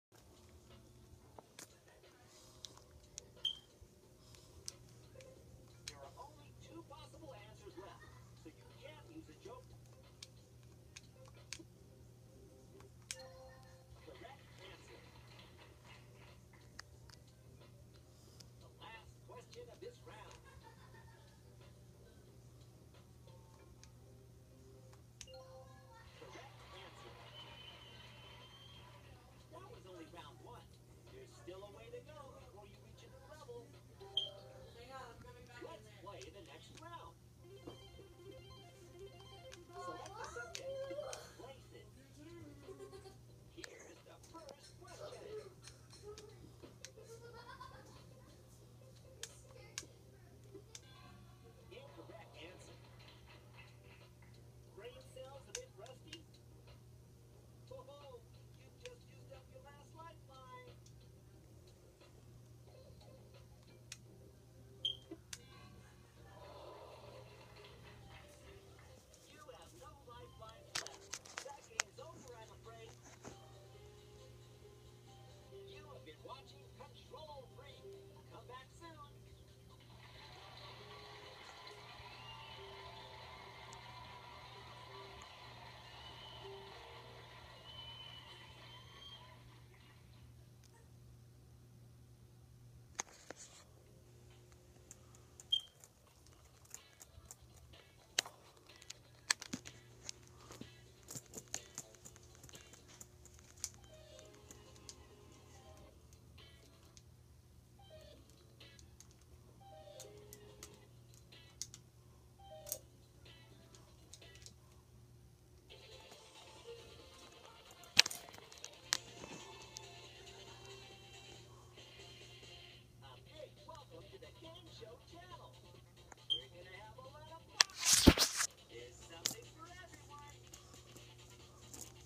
Daithan On Family Gameshow (hosted by Larry Lightfoot!
Daithan... Come on down! You are the next player on Control Freak!